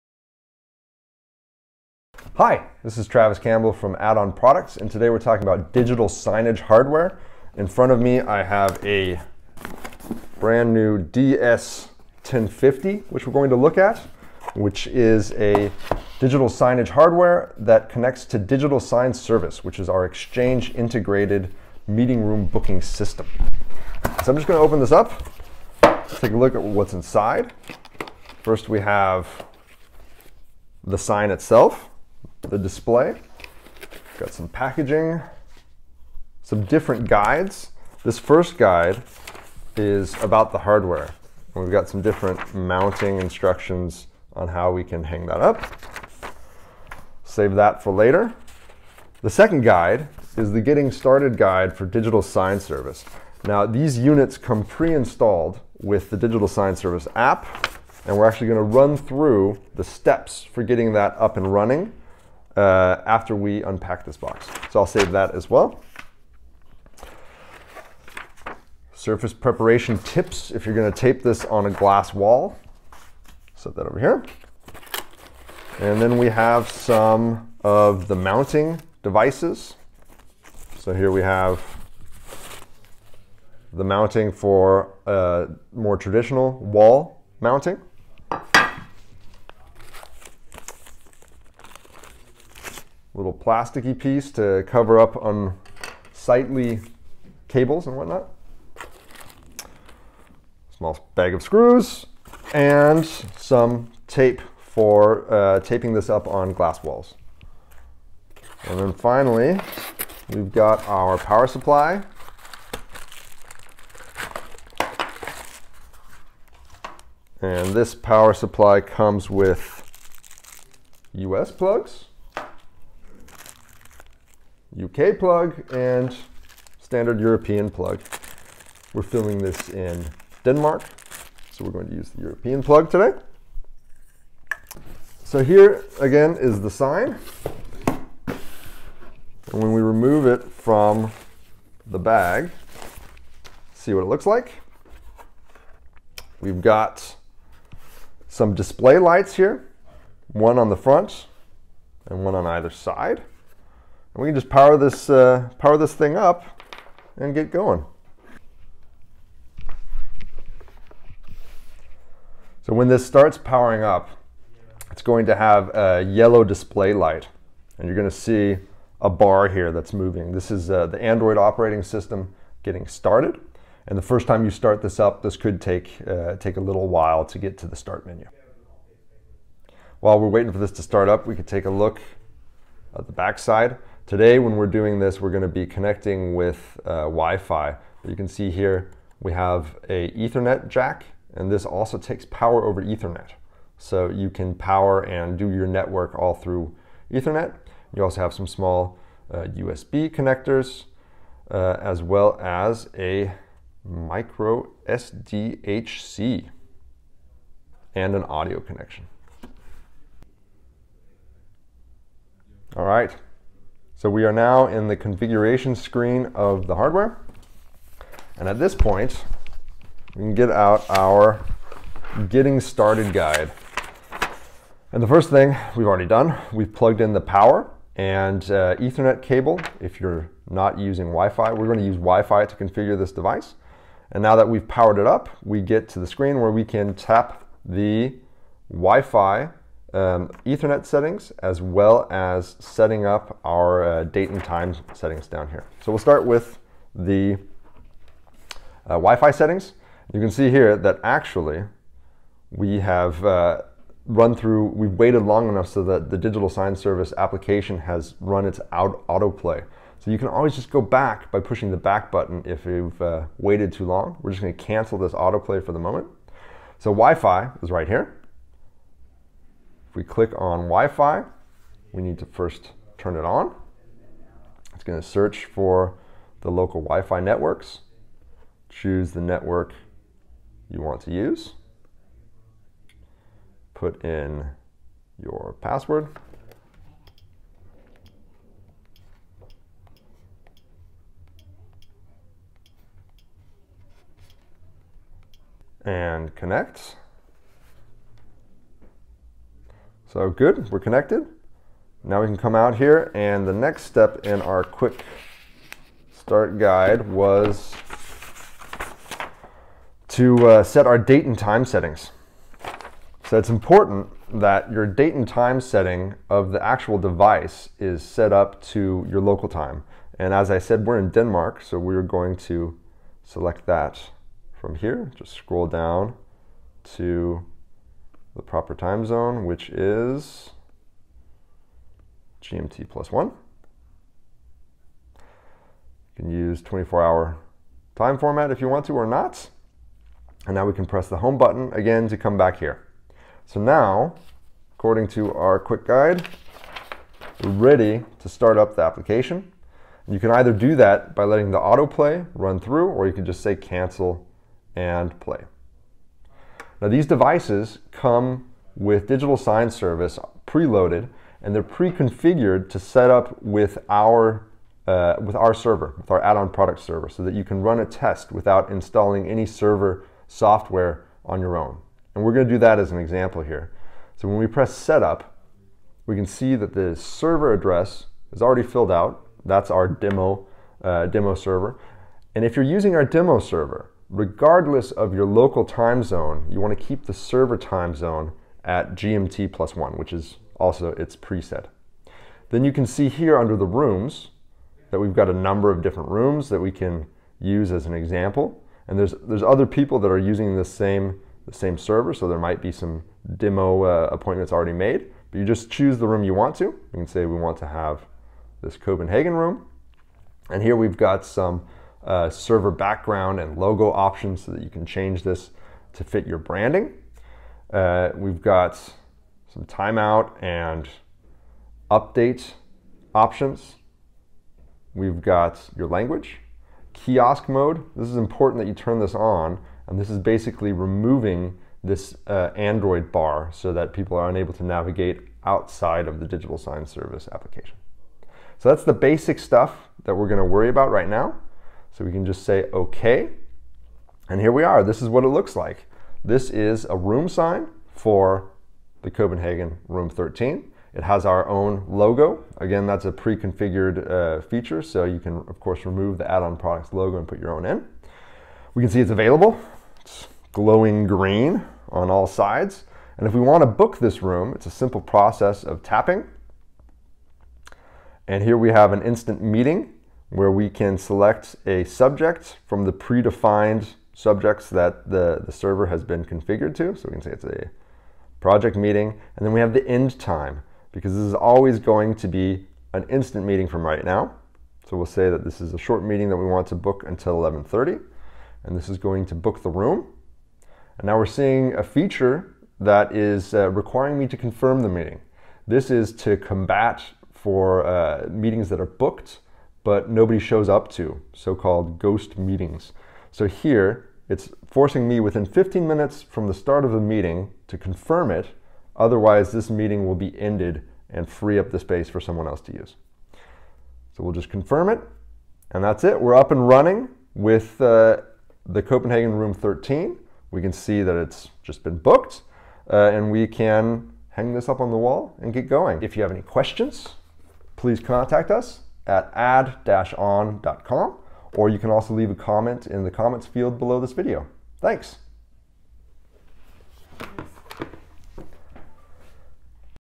Meeting Room Booking Digital Sign Display for Microsoft Outlook, Office 365 and Microsoft Exchange
Looking for a meeting room booking digital door sign display for Microsoft Outlook, Exchange or Microsoft 365? Take a look at this product demonstration where we unbox aDS-1050, Add-On Product's room booking sign for Microsoft 365, Microsoft Outlook and Microsoft Exchange.

* Display availability and book a room right at the door.
* Customize the layout to fit with your branding and provide your users with a feature rich meeting room booking door sign for Microsoft Exchange, Microsoft 365 and Microsoft Outlook.
* Enable our confirm meeting feature requiring users to confirm the meeting shortly before or after the meeting start.
* If the meeting is a no show, the meeting room booking door sign for Outlook, Exchange and Microsoft 365 will end the meeting and free up the room for others to use.